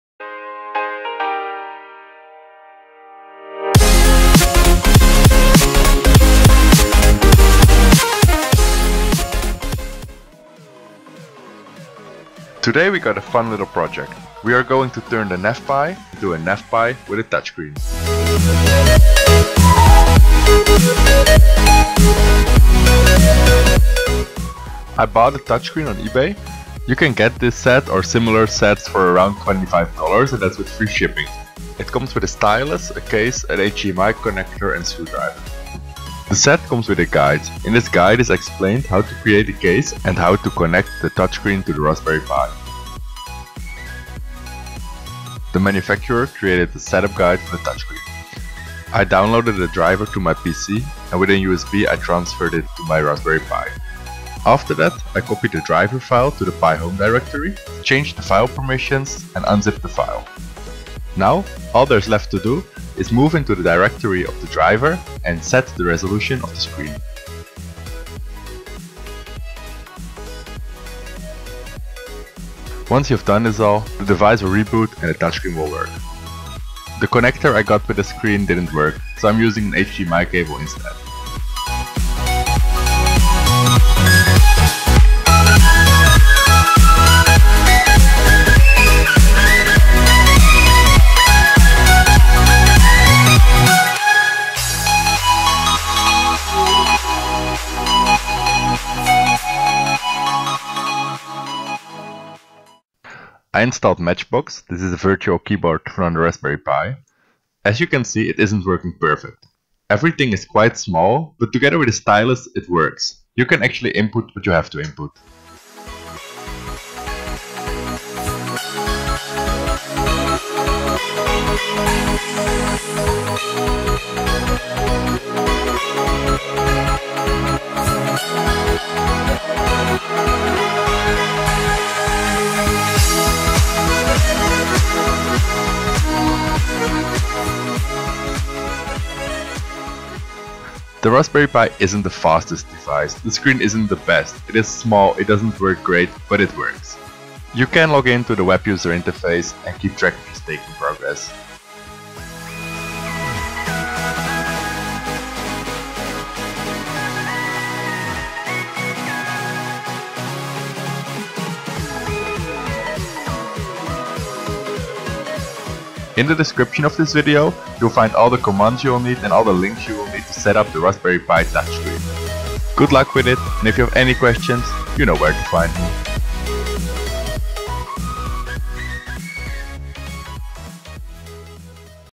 NavPi Touch Screen (Nav Coin Stakebox)
1. Download Driver to your PC
2. Copy the driver and paste it to the home folder of raspberry pi (/home/pi)
3. Open the command line
4. Change permission: “sudo chmod 777 LCD_show_35hdmi.tar.gz”
5. Unzip the file: “tar -xzvf LCD_show_35hdmi.tar.gz”
6. Enter the driver directory: “cd LCD_show_35hdmi”
7. Select the Resolution (I recommend 810*540): “sudo ./LCD35_810*540”
8. Wait for the Pi to reboot

Touch Screen (Ebay)